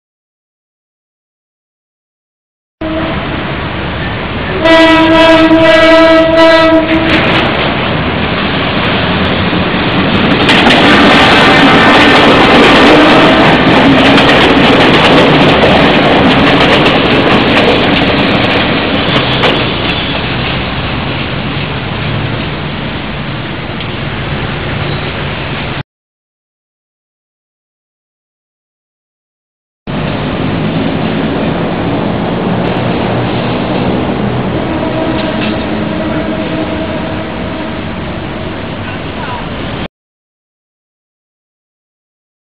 Kiha 59 Kogane Passing Pasay Road Railroad Crossing
Kiha 59 Arrives @ Pasay Road Station for Loading for possible Passengers Bound for Ligao,Albay